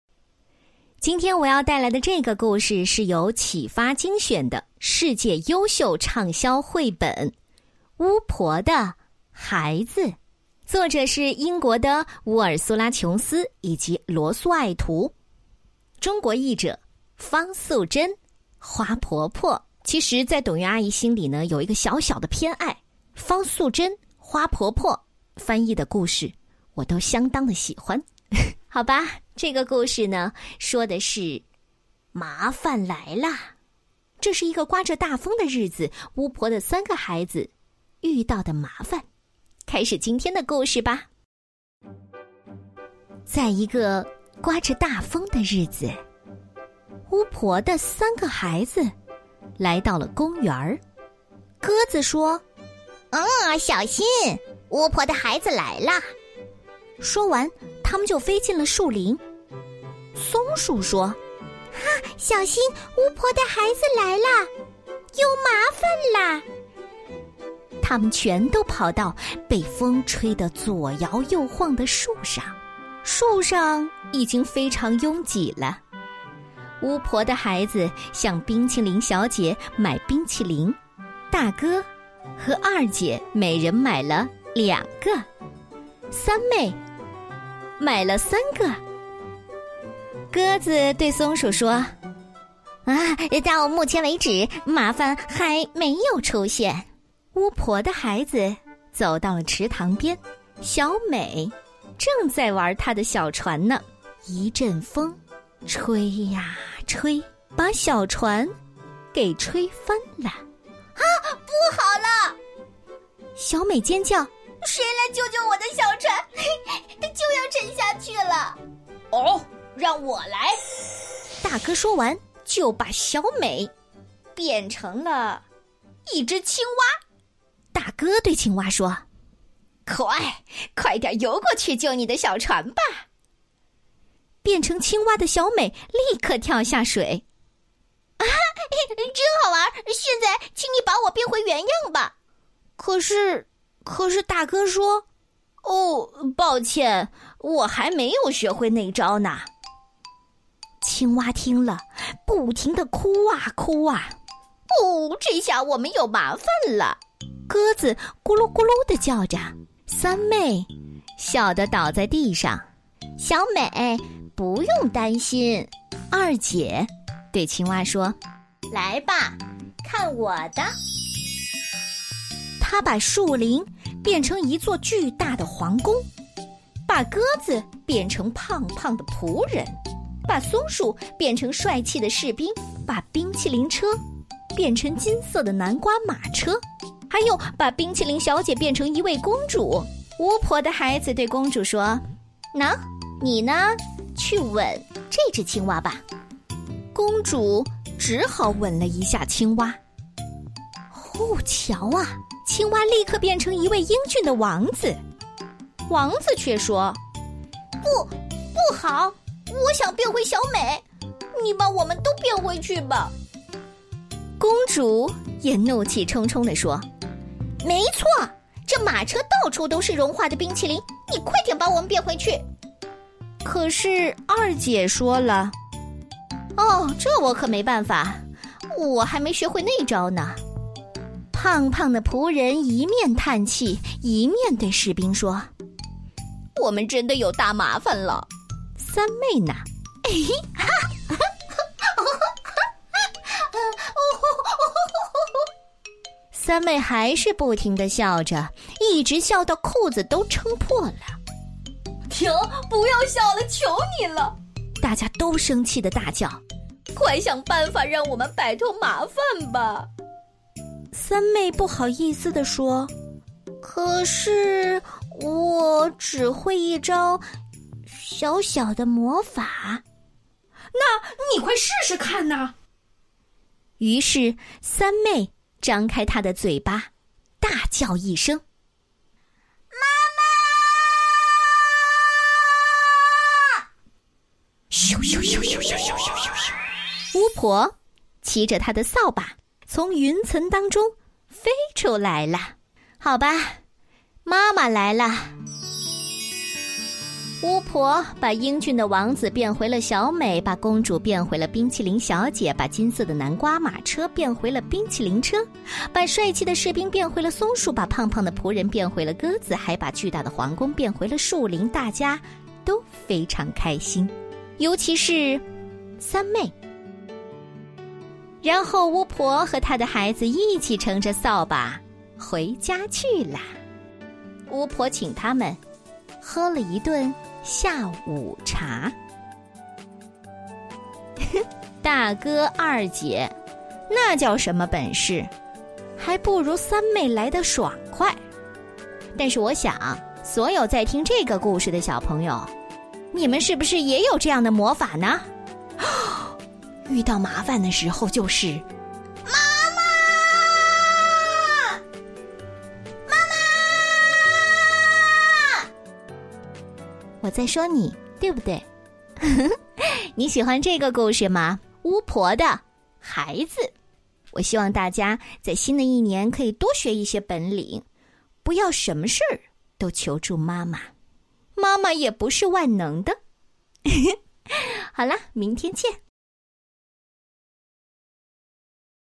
【董乐讲故事】：巫婆的孩子
宝宝加油
宝宝加油
微信号 bbjyikidio
巫婆的孩子和普通的孩子一样，喜欢在免除了成人权威的同伴中间，展示自己的本领，虽然有时候也会不自量力，炫耀能耐之后却无法收拾残局。女孩小美变成了青蛙，青蛙变成了王子，王子怎么变回小美呢？被魔法折腾的伙伴们，如何恢复真身呢？可真让人着急啊！这样一个单纯的快乐的故事，给予孩子情绪愉悦的满足，也帮助孩子不经意间完成自我教育。
特别鸣谢：河北教育出版社 北京皮卡中英双语图书馆
阅读
在看
已同步到看一看
取消 发送
我知道了
已发送
发送中
微信扫一扫